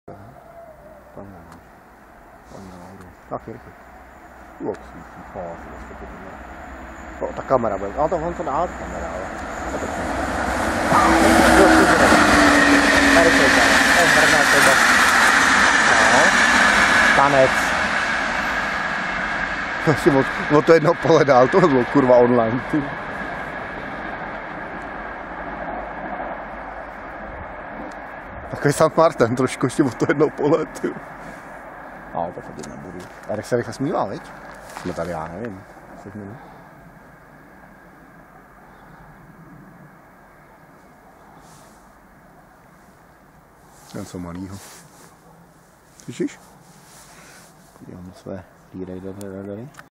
Rychlík českých drah
Czech railways train passing Poříčany Station near Prague